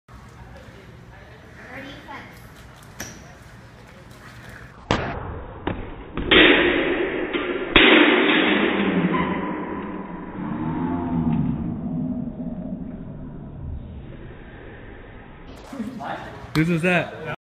Fencing Friday with BeamIt!
"Ready - Fence!" Enjoy this short slo-mo video of our first official FencingFriday - BeamTeam mates Bryson and Adam rocking it!